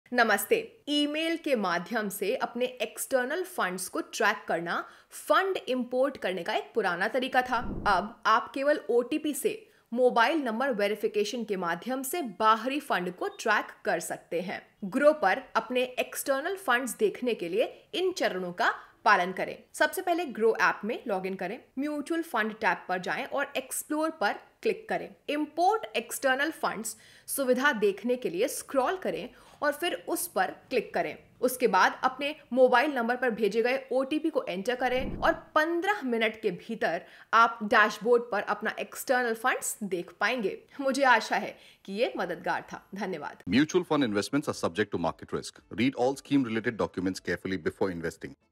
बाहरी म्यूच्यूअल फंड्स Groww app पर नहीं दिख रहे , क्या करें? I Groww app kaise use kareinI
बाहरी म्यूच्यूअल फंड्स Groww app पर नहीं दिख रहे , क्या करें? I Groww app kaise use kareinI Get to know Groww

You can choose or type in from the categories to find a solution for your query.